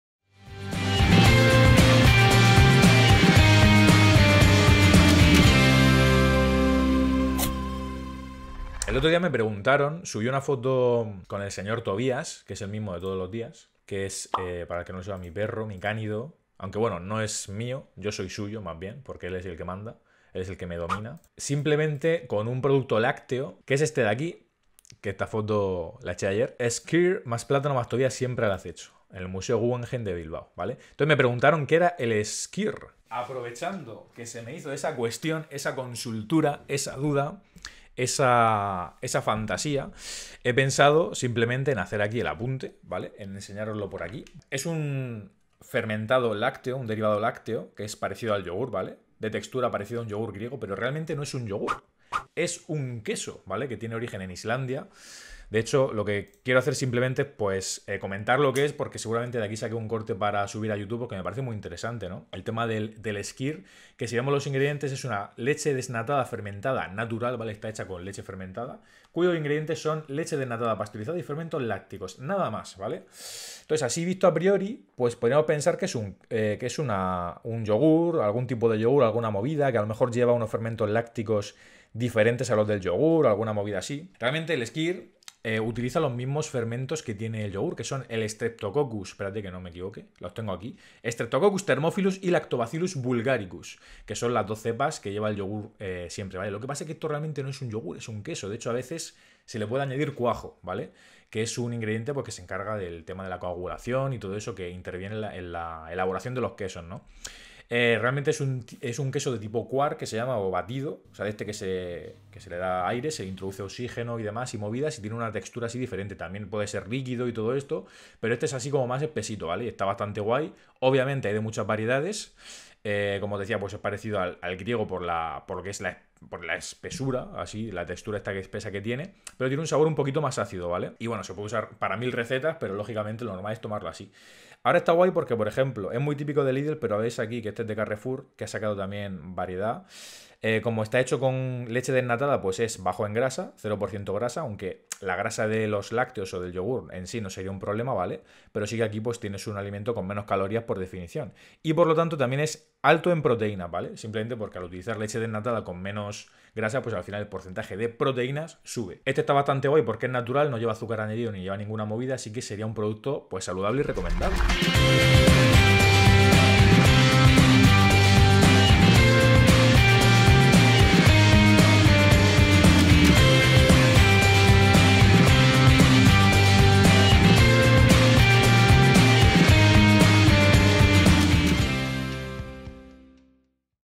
¿QUÉ ES EL SKYR? | Descubre esta fantasía láctea | SefiFood
El skyr es un derivado lácteo que se elabora con los mismos fermentos que el yogur: Streptococcus thermophilus y Lactobacillus bulgaricus. A pesar de ello, el skyr no es realmente un yogur sino un queso. Elaborado con leche desnatada y alto en proteínas, el skyr resulta una opción muy interesante para incluir en la dieta por ser bastante saciante a la par que saludable.

PD: los procesos de elaboración no son los mismos para queso y yogur. Muy resumidamente: para hacer yogur se produce una fermentación bacteriana de la lactosa (el azúcar de la leche), mientras que para hacer queso tiene lugar la coagulación de la caseína (una proteína de la leche).